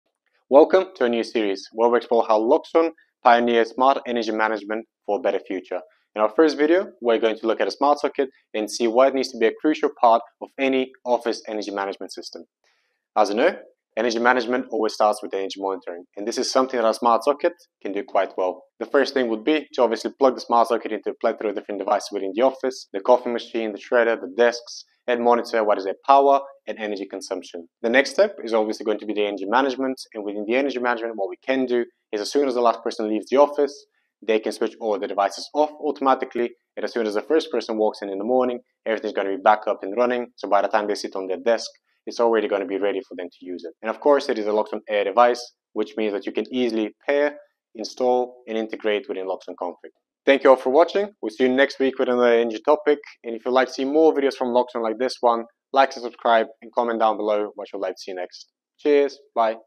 Loxone - Pioneering Energy Management - Smart Sockets in commercial buildings
Niko talks us through how useful the Loxone Smart Socket is, within the wider Loxone building management system, for energy management. Its implementation in commercial spaces makes for a fast ROI, saving the business money. Ultimately, helping businesses to be more green and hit sustainability targets.

We have prepared the following documentation for our Loxone installers on the topic